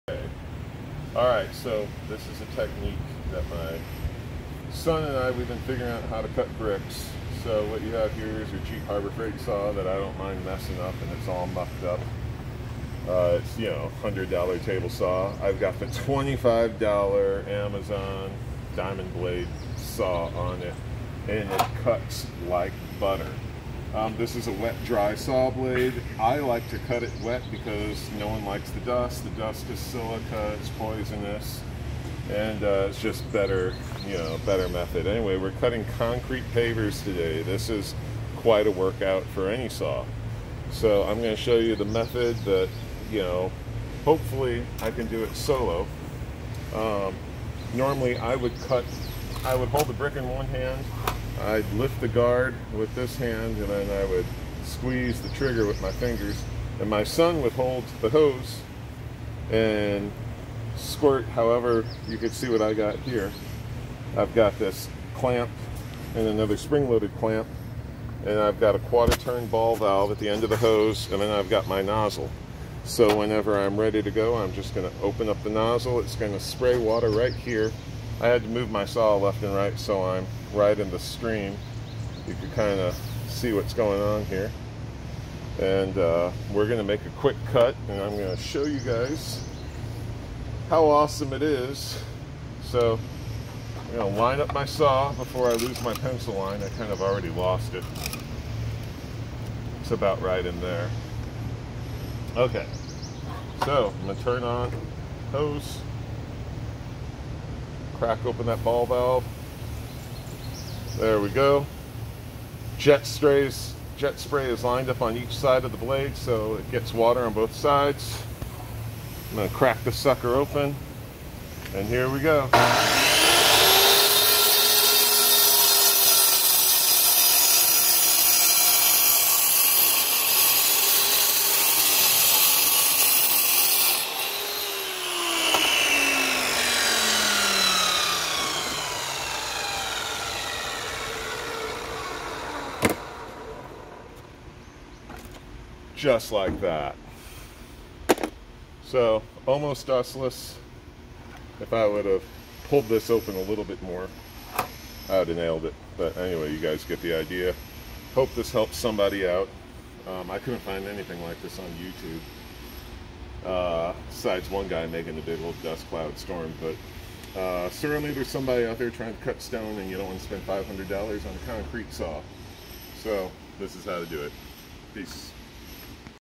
how to cut pavers easy, cheap and dust free
Chicago electric miter saw cutting pavers